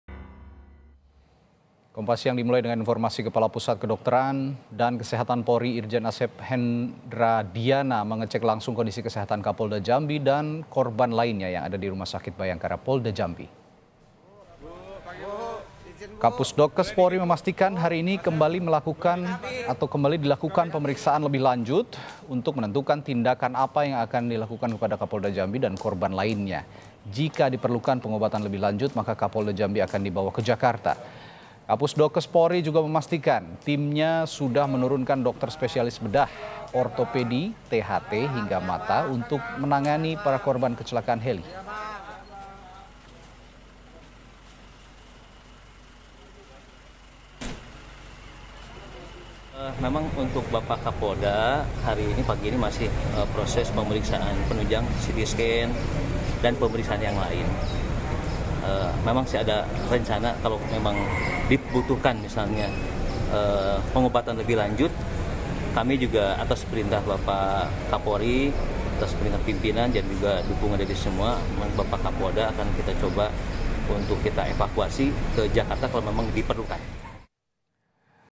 Pusdokkes Polri Periksa Ulang Kondisi Kesehatan Kapolda Jambi dan Rombongan
JAMBI, KOMPAS.TV - Kepala Pusat Kedokteran dan Kesehatan Polri, Irjen Asep Hendradiana mengecek langsung kondisi kesehatan Kapolda Jambi dan korban lainnya yang ada di Rumah Sakit Bhayangkara Polda Jambi.

Kapusdokkes Polri memastikan, hari ini (22/2) kembali dilakukan pemeriksaan lebih lanjut untuk menentukan tindakan apa yang akan dilakukan kepada Kapolda Jambi dan korban lainnya.

Jika  diperlukan pengobatan lebih lanjut, maka Kapolda Jambi akan dibawa ke Jakarta.

Kapusdokkes Polri juga memastikan, timnya sudah menurunkan Dokter Spesialis Bedah, Ortopedi, THT, hingga Mata untuk menangani para korban kecelakaan heli.

Lantas, bagaimana kondisi korban kecelakaan helikopter di Kerinci yang telah dirawat di Rumah Sakit Bhayangkara Jambi?

Adakah cerita di balik evakuasi rombongan Kapolda Jambi?

Sudah ada Nandha Aprilia, Jurnalis Kompas TV.